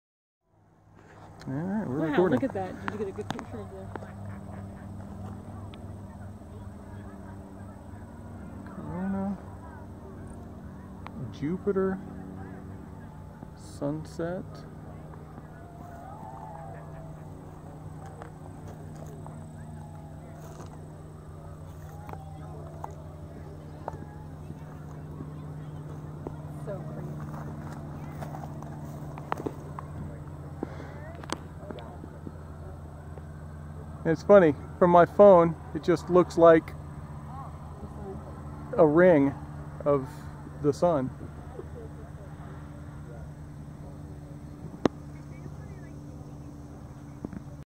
Solar Eclipse Totality 360*
August 21, 2017 Total solar eclipse outside Hopkinsville, KY at 1:24pm.  We got 2:40 of totality